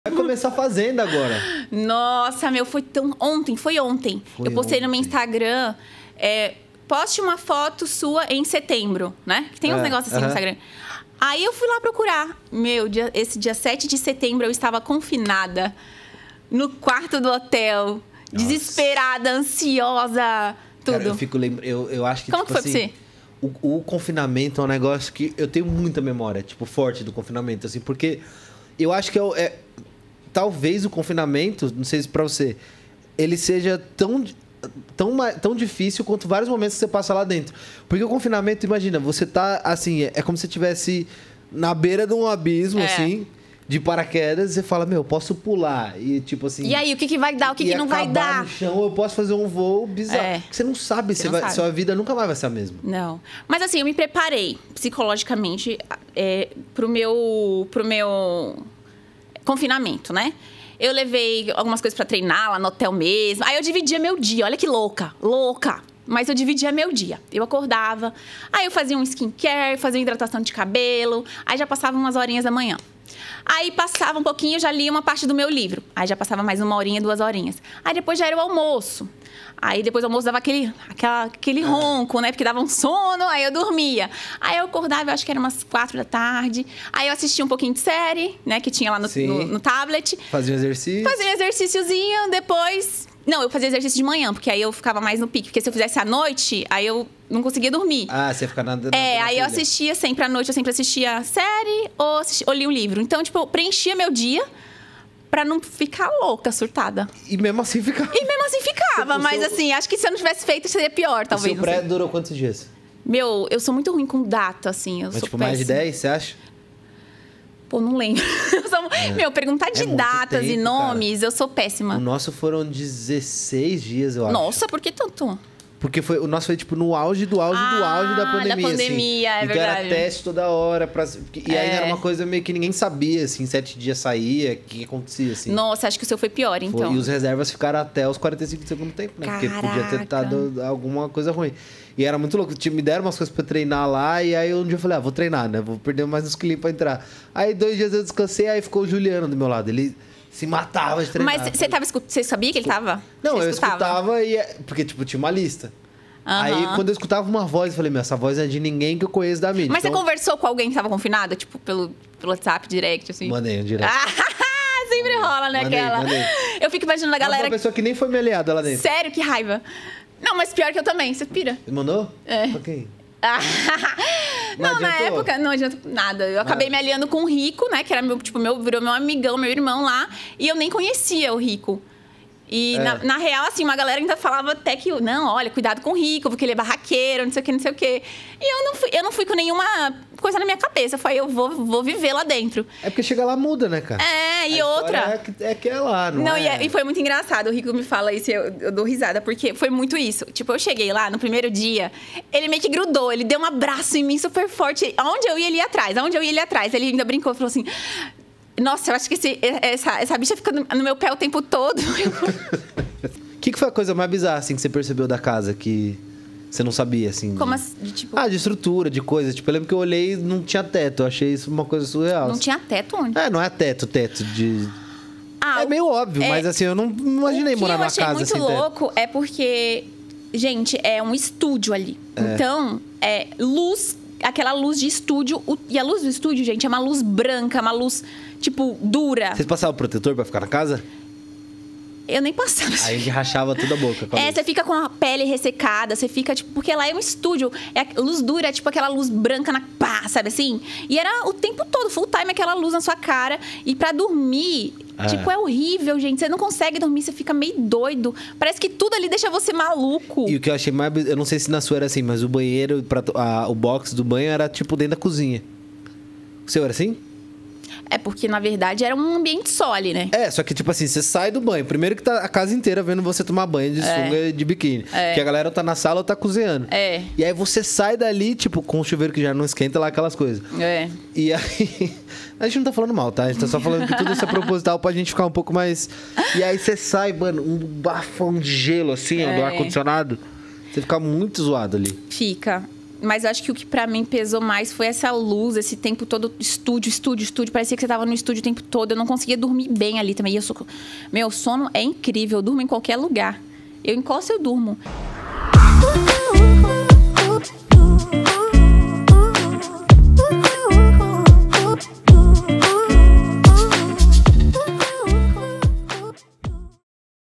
A ROTINA DE UM CONFINADO ANTES DA FAZENDA | ERIKA SCHNEIDER | LINK PODCAST
Na contagem regressiva para a próxima temporada de “A Fazenda”, Lucas Selfie entrevista a ex-peoa Erika Schneider. A dançarina conta como foi o período de confinamento no hotel.